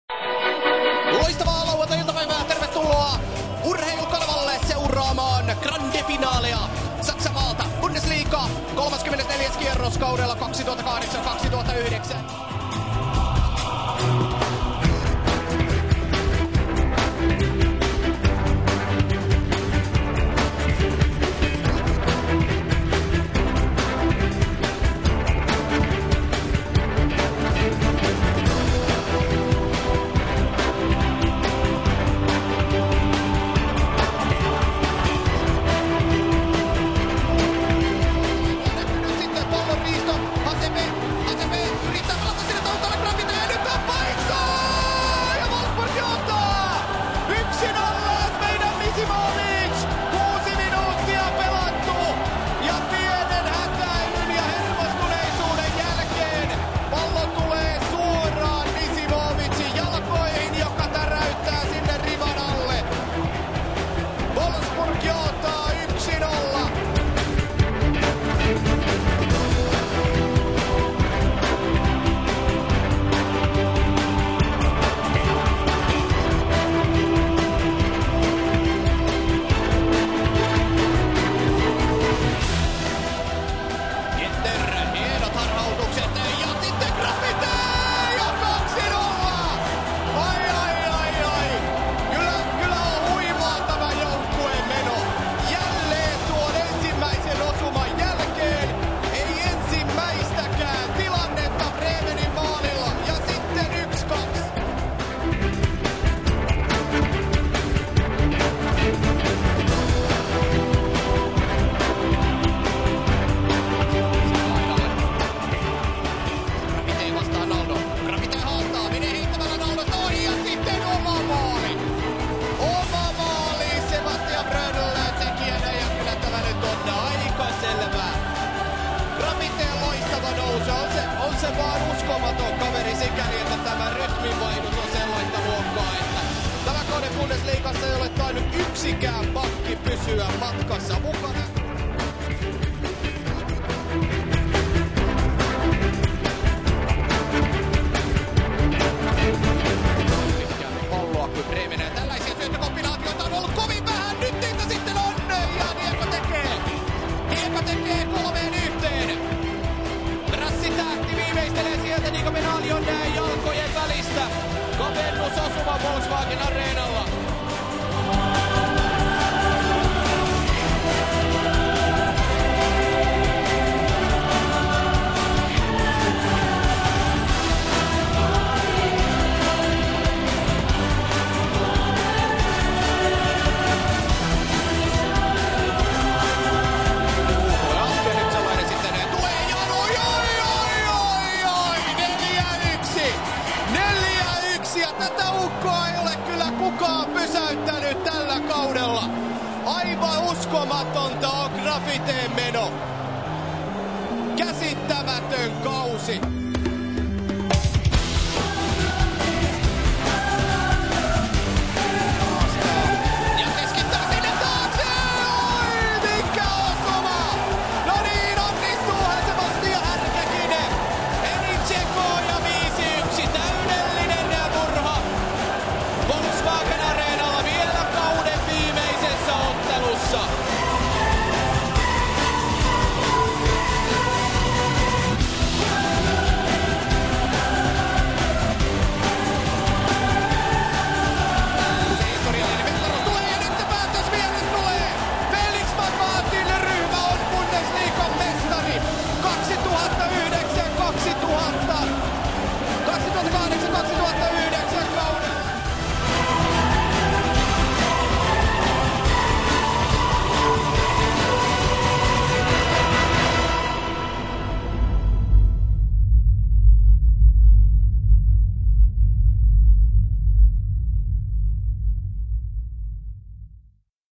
Urheilukanavan viimeinen Bundesliiga-peli
Jotain pientä fiilistelyä, kun tuo peli löytyi digiboksin kätköistä.